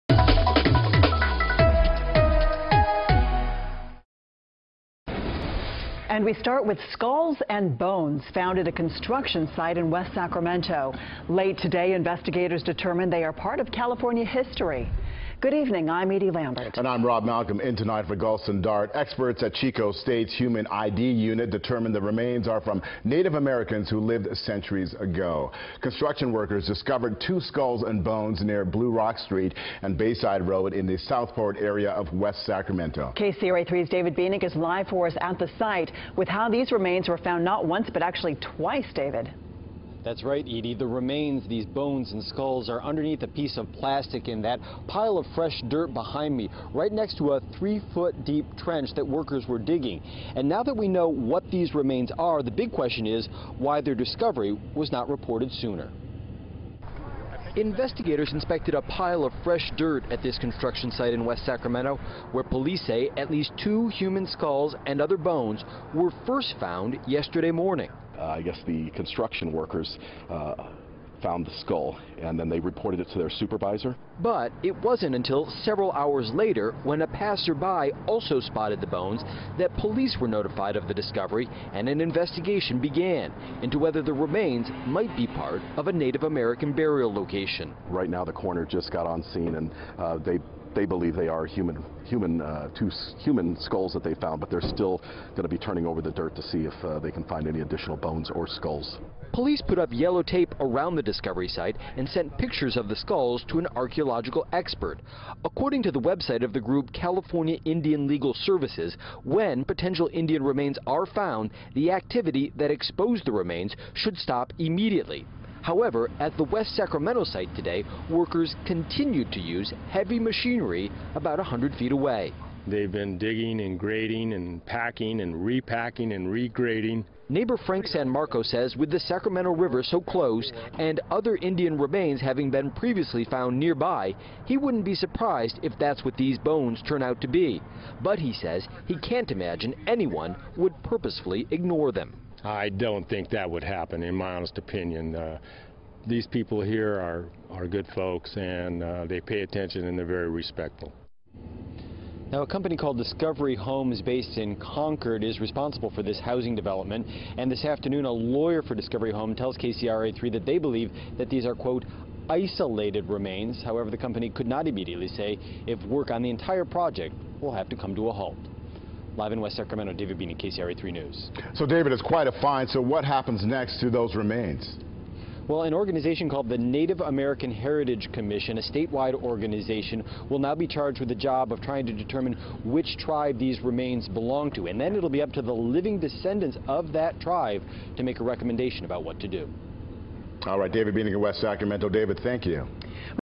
Historic bones found at West Sacramento building site
Two skulls and bones found at a construction site are the remains of centuries-old Native Americans.